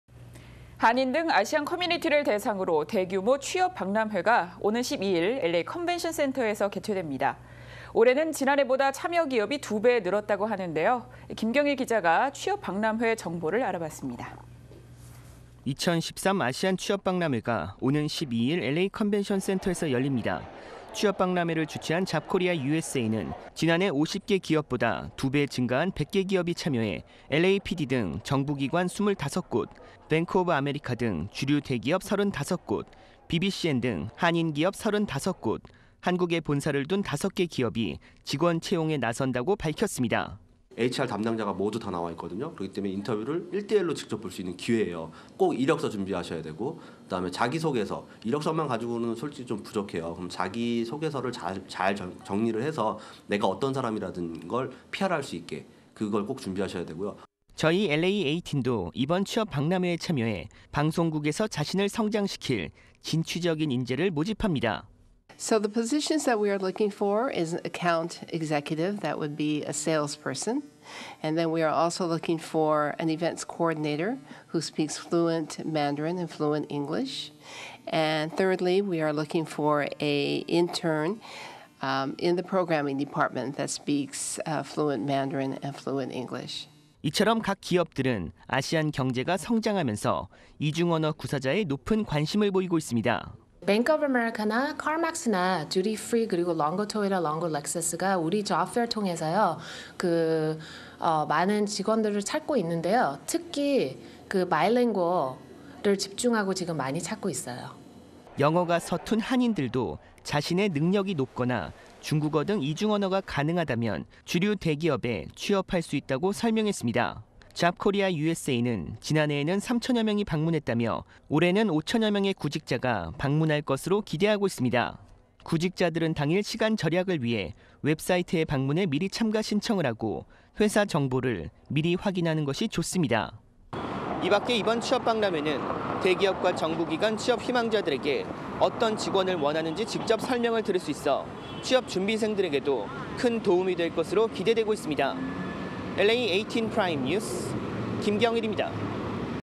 040413 - 대규모 아시안 취업박람회 LA18
아시안 위한 대규모 취업박람회
다음주 LA 컨벤션 센터